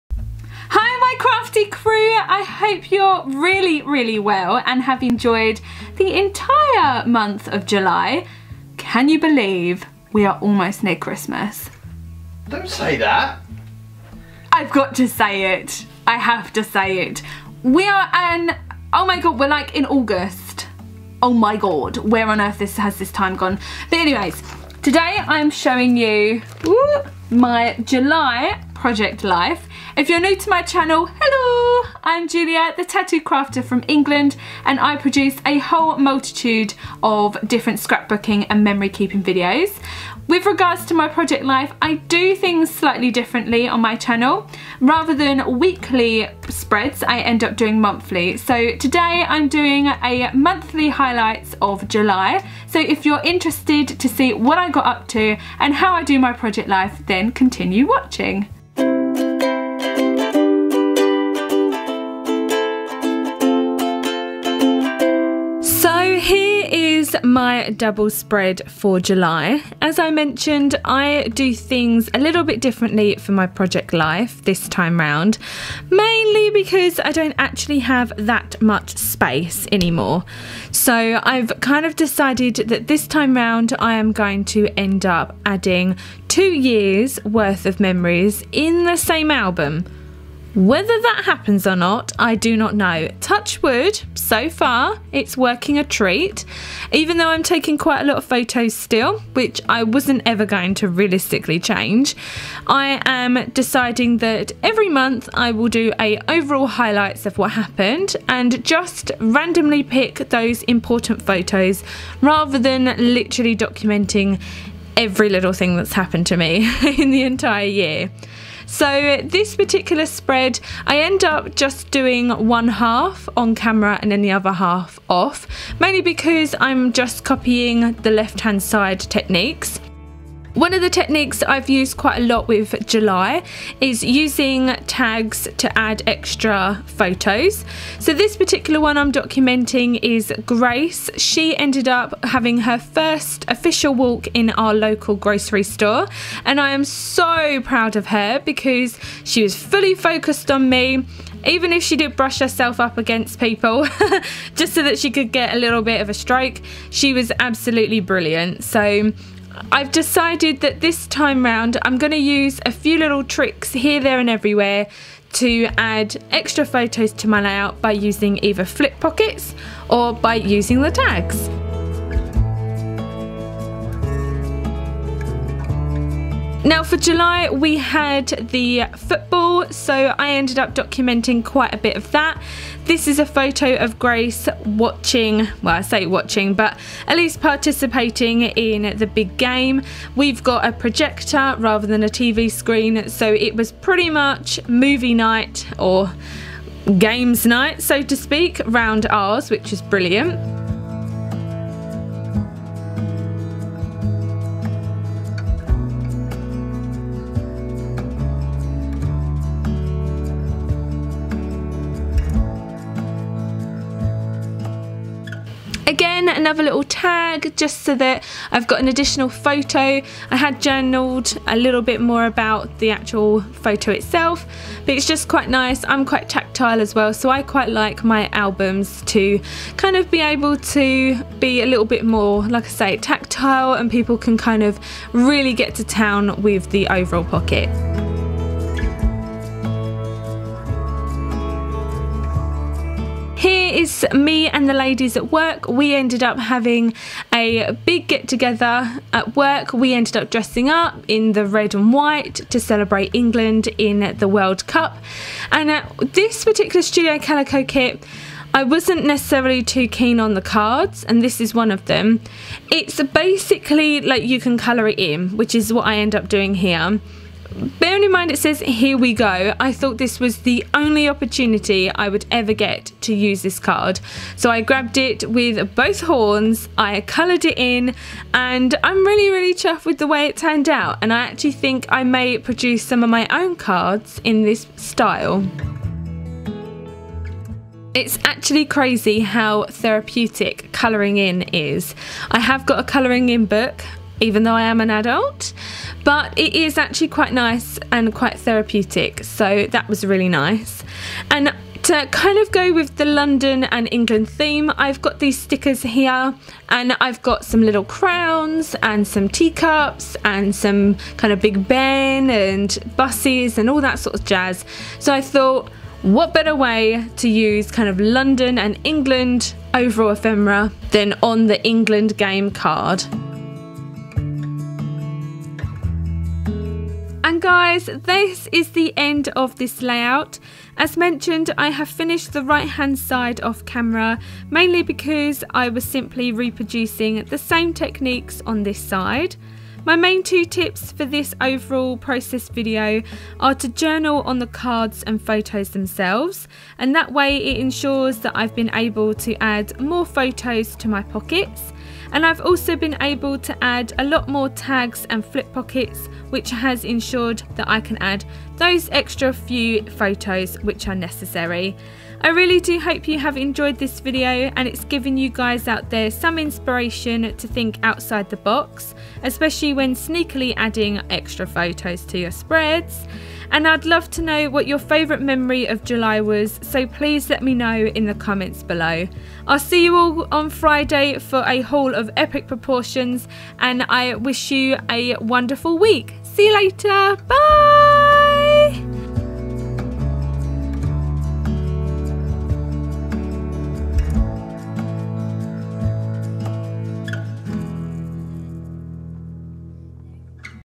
CREATING MONTHLY PROJECT LIFE SPREADS - JULY HIGHLIGHTS
Documenting my life in a different way using Project Life. This year, instead of weekly spreads, I am creating monthly highlights to document a few adventures that month, in the hope that i will be able to gain 2 years of memories in the same album.

I post a variety of Project Life, 12 x 12 scrapbooking and Journal videos, every Tuesday & Friday @ 12 pm GMT time.